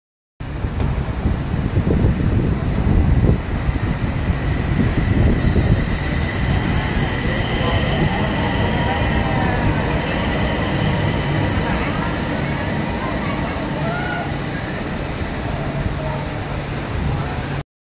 バンコクBTS 中国中車南京浦鎮車輛製 ボンバルディアInnoviaAPM300 ゴールドライン クローンサーン駅到着 2024/2/28
BTS(Bangkok Train Service)
Bombardier Innovia APM300 made by Nanjing Puchin Vehicles in China
Arrival at Khlong San Station on Gold Line